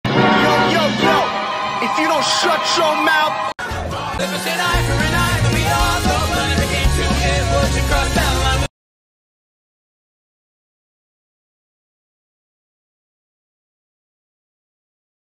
mini wip/artfight dump
first one is a portion of a collab I’m doing with @AkimboACA
second one is a tiny section of a collab map part I’m doing
thirds a map part that j really need to finish or drop
last is some artfight attakjs jbe done ive done a lot more but i didn’t like them enough to show them here 😭

follow me on Artfight I do guaranteed revenges to anyone who attacks